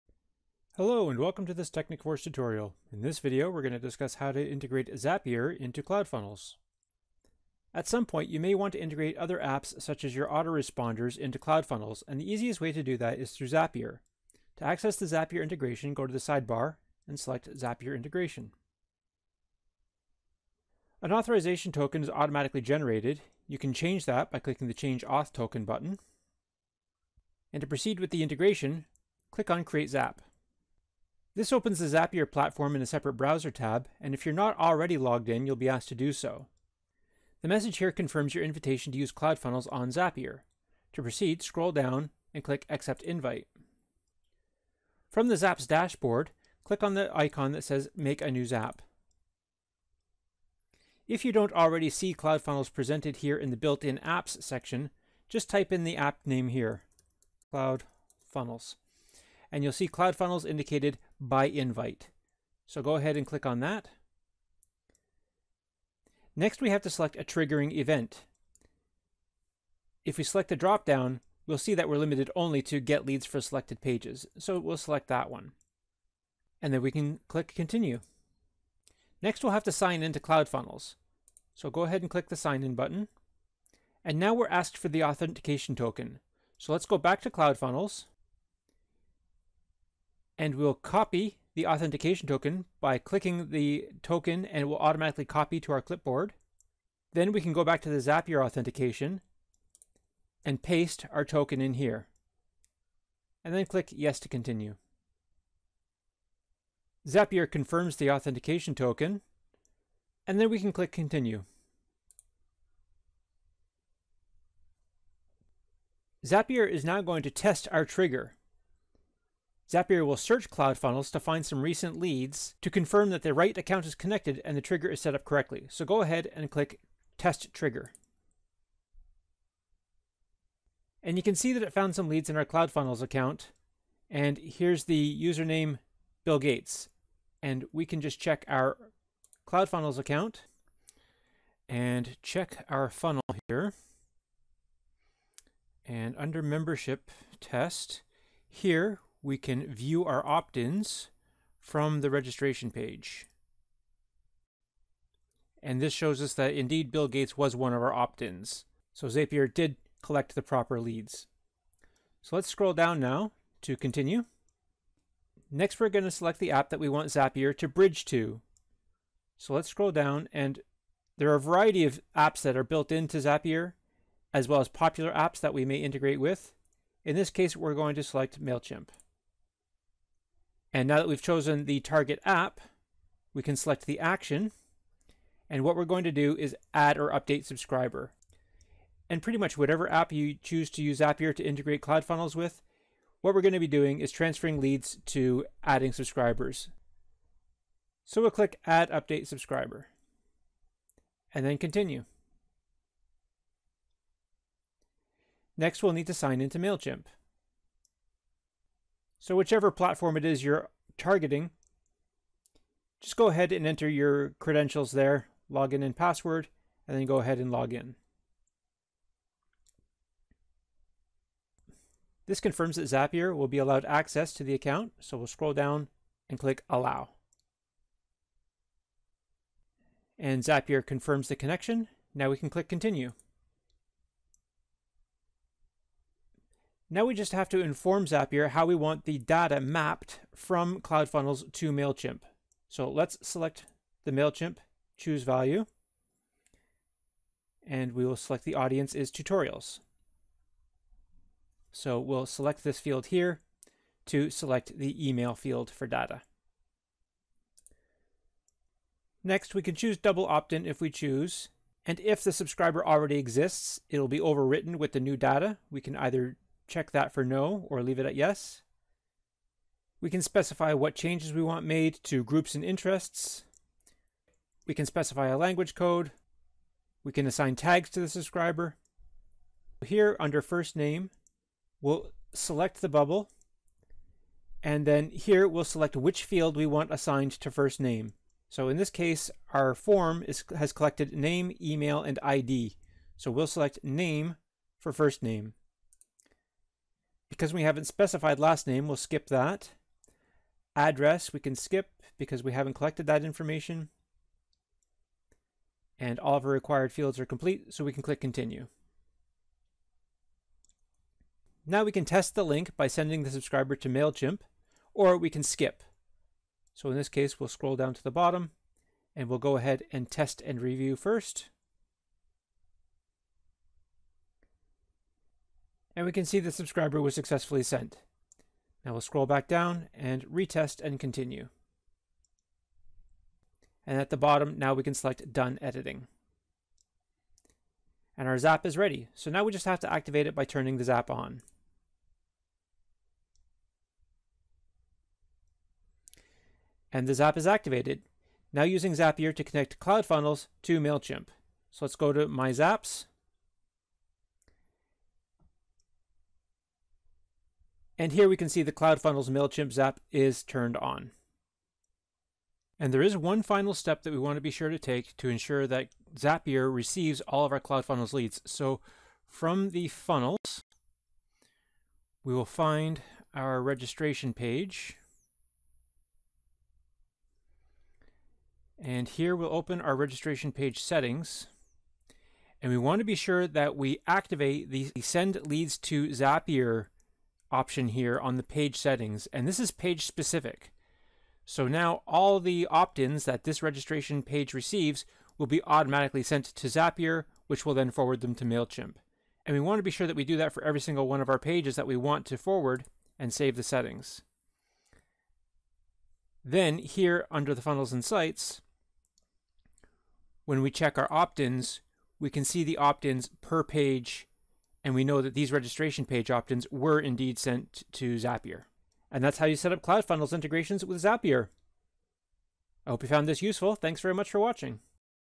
Cloudfunnels - Zapier tutorial
You may want to integrate other Apps such as Autoresponders into Cloudfunnels. The easiest way to do this is through Zapier.

In this video we are going to take you through the steps required to integrate Zapier with Cloudfunnels.